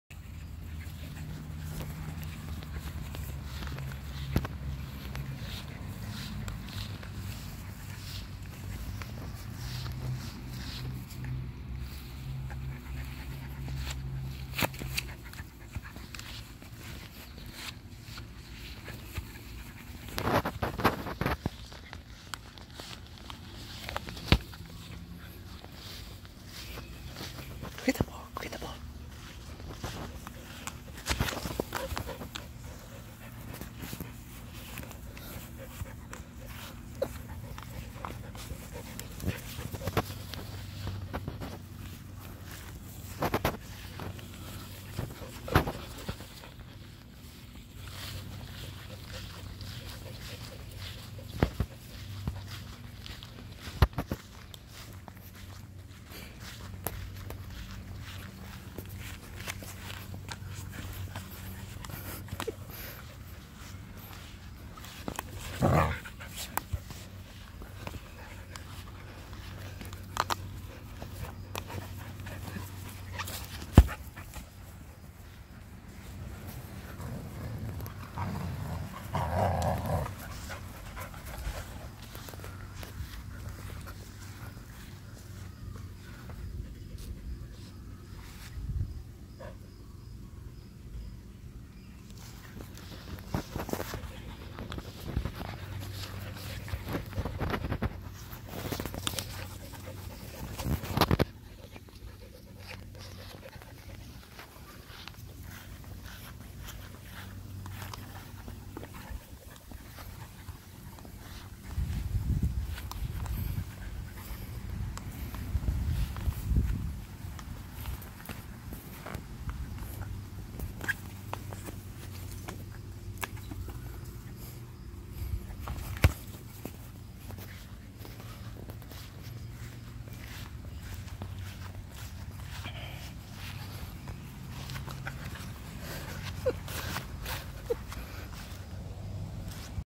Samoyed and Keeshond - Football Phenom Keeshond Puppy
He's only 13 weeks old, but young Zuko is already showing flashes of soccer stardom!